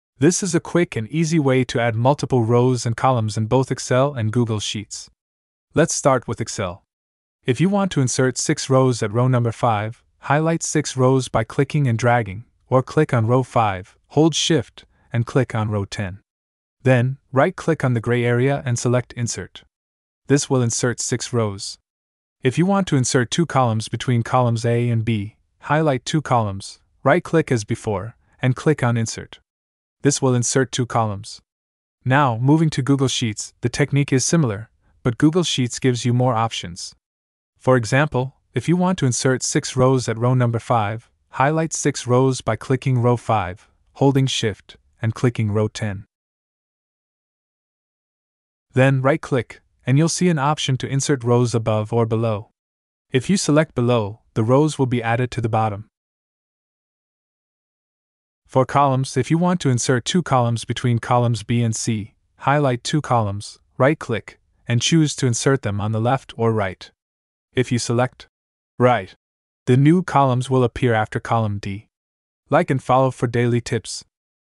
Add MULTIPLE Rows and Columns in Excel and Google Sheets in SECONDS!
Learn how to add multiple rows and columns in Excel and Google Sheets quickly and efficiently! In this tutorial, we'll show you the fastest way to insert multiple rows and columns in your spreadsheets, saving you time and increasing your productivity. Whether you're a beginner or an advanced user, this simple trick will revolutionize the way you work with Excel and Google Sheets. So, if you want to add multiple rows and columns in seconds, watch this video now and become a spreadsheet master!